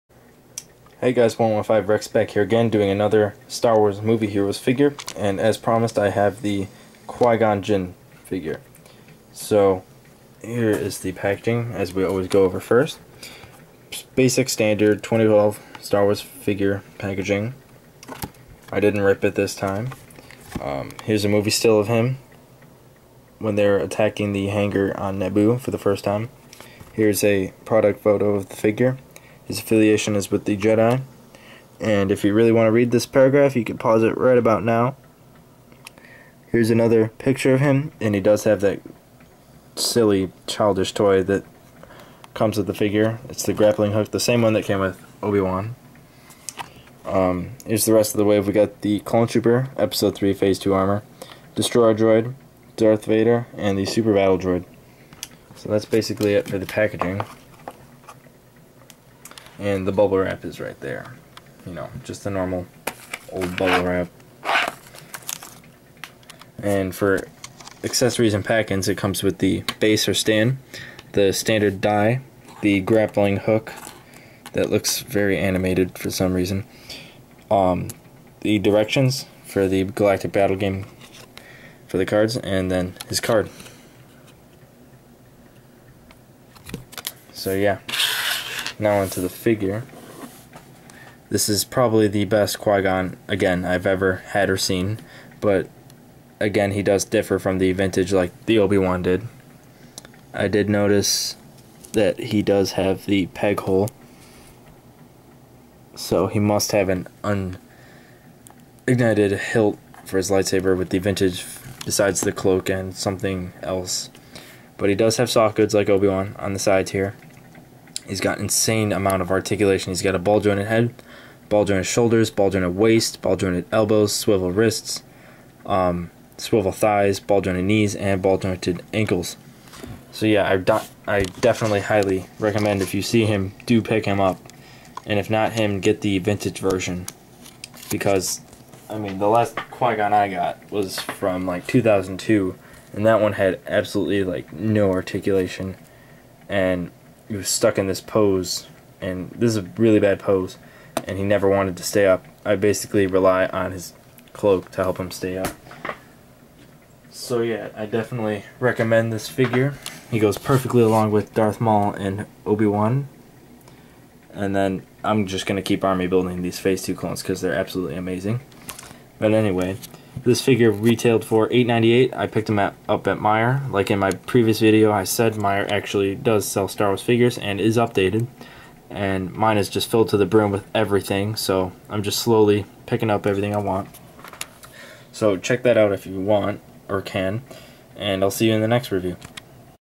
Star Wars Movie Heroes Qui Gon Jinn figure review
This is Qui Gon from the Phantom Menace and he is very articulate and I highly recommend him to your Jedi collection or your Phantom Menace collection. He retails for $9.98.